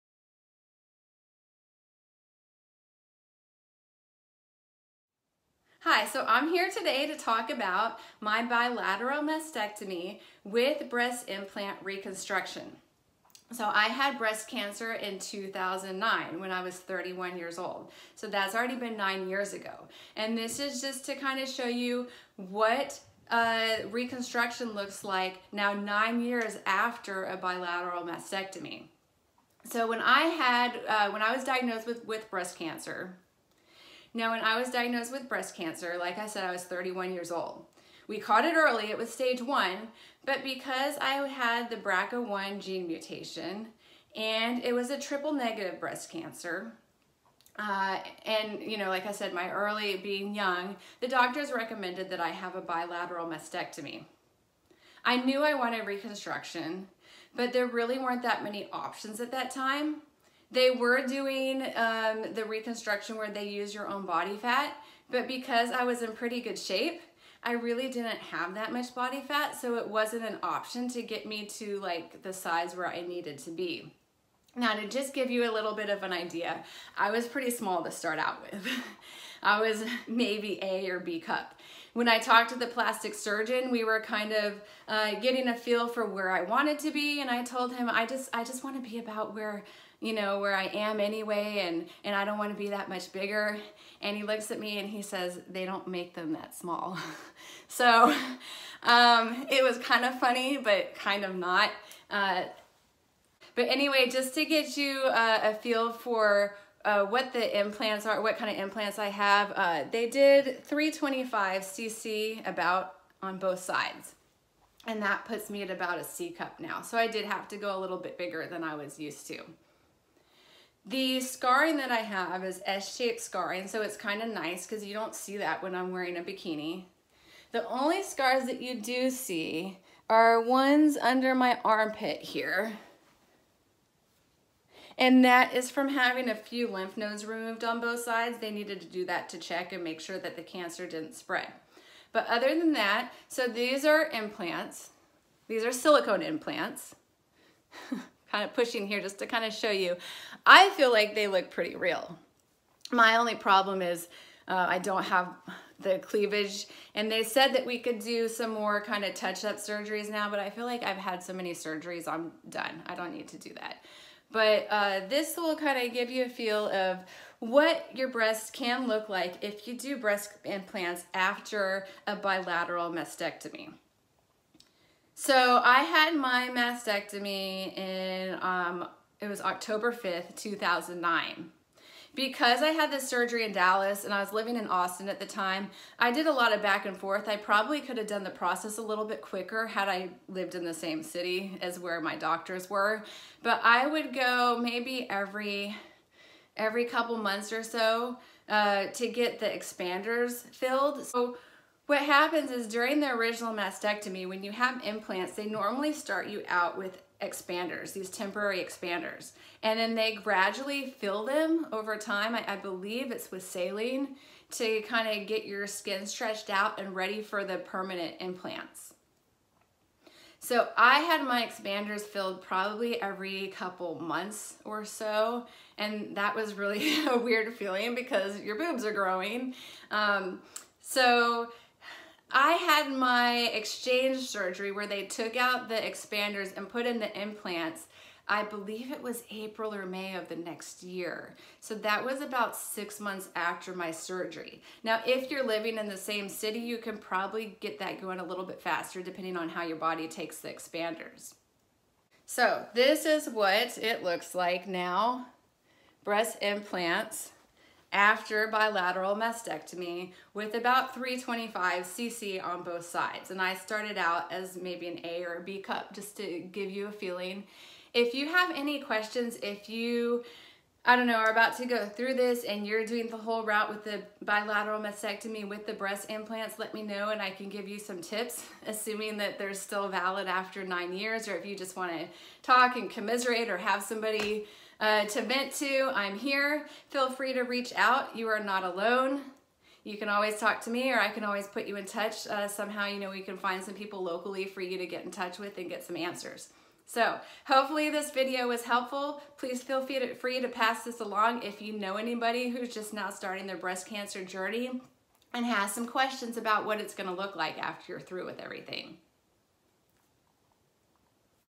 Bilateral Mastectomy with Breast Implant Reconstruction - 9 Years Later Update
As a young breast cancer survivor with triple negative breast cancer and BRCA positive, I had a bilateral mastectomy with breast implants reconstruction in 2009.  Here’s an update 9 years later. (Video from 2018).